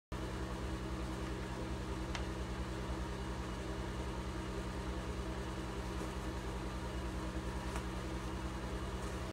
Nico, The Aries Kitty, Gone Wild
Nico was already in bad kitty time out back in his room and then I walked in to find him doing this... nicogoeswild.